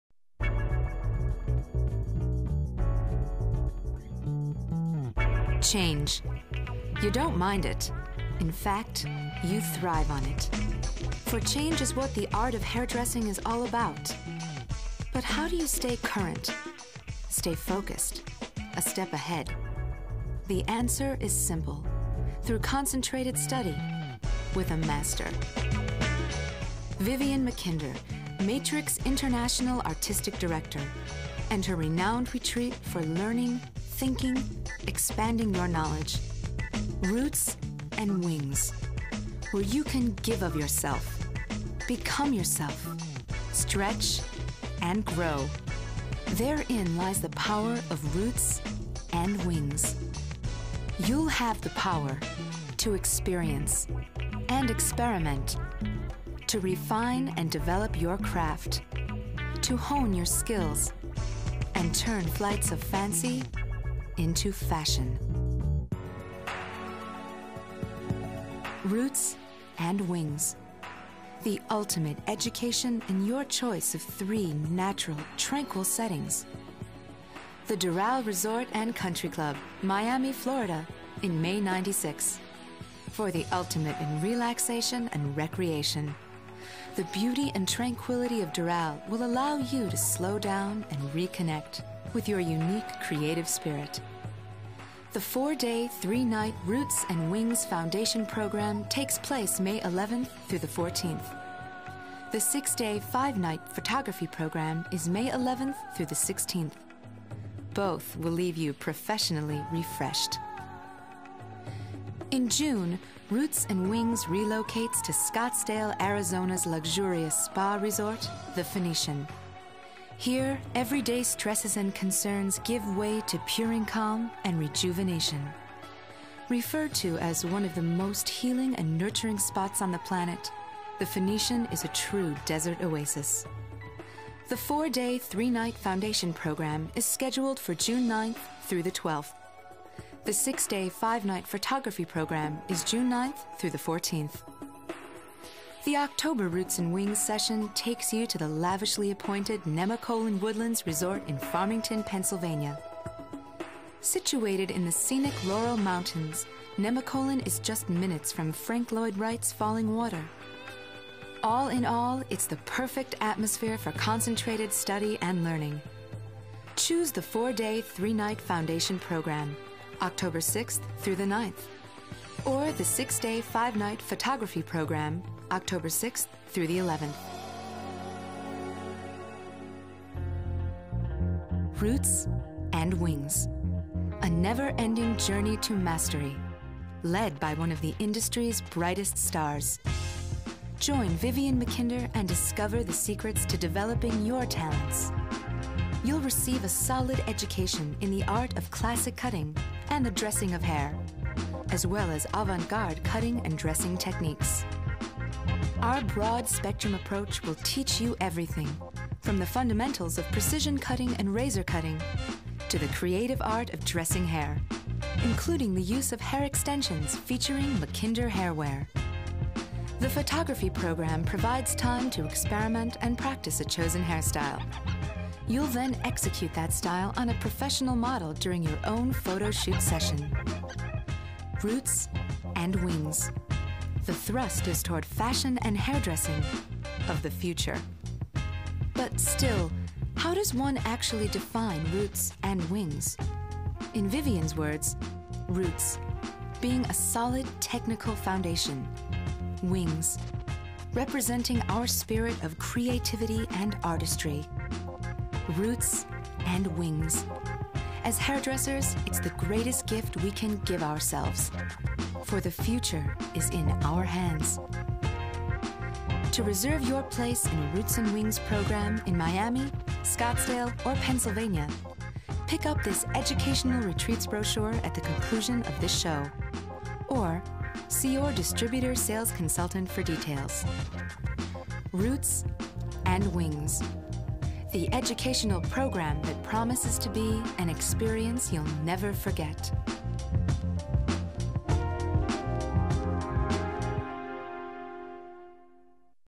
Roots & Wings Promo 1996
A video promo for a hairdressing seminar given by Vivienne MacKinder at a spa location in 1996.
Running time is 5:55